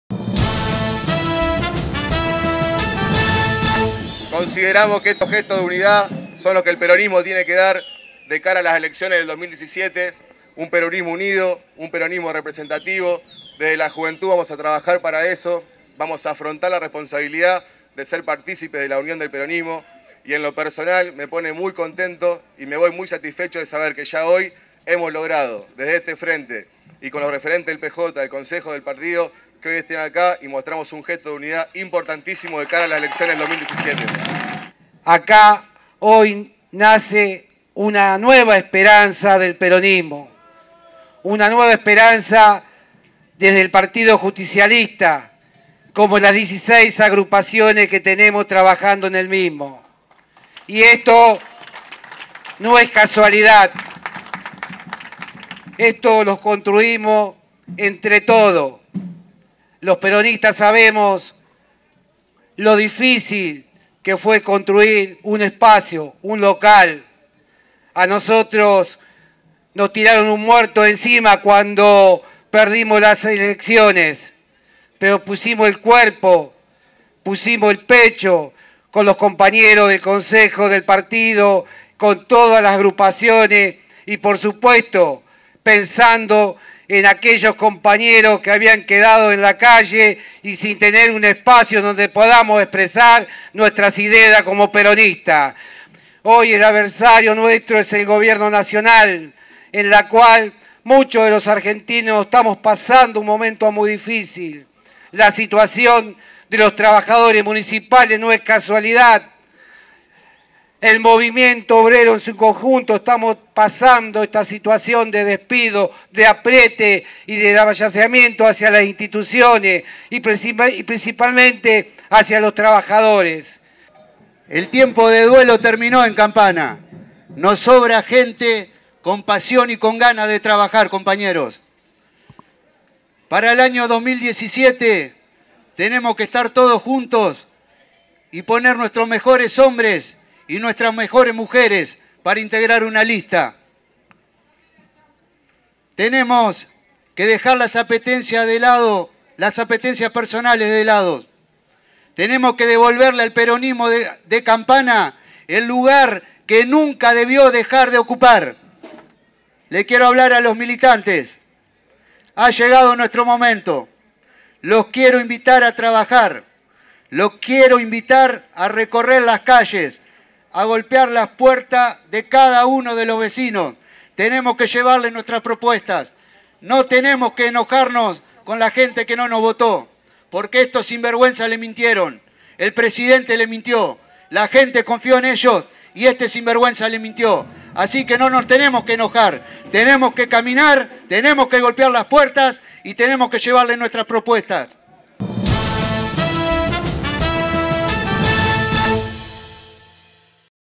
Frente vecinos por Campana inuguró su sede
El viernes quedó inaugurada la sede partidaria del Frente Vecinos por Campana.  El acto contó con la presencia de referentes del peronismo local.  El espacio político busca representar al justicialismo en las próximas elecciones.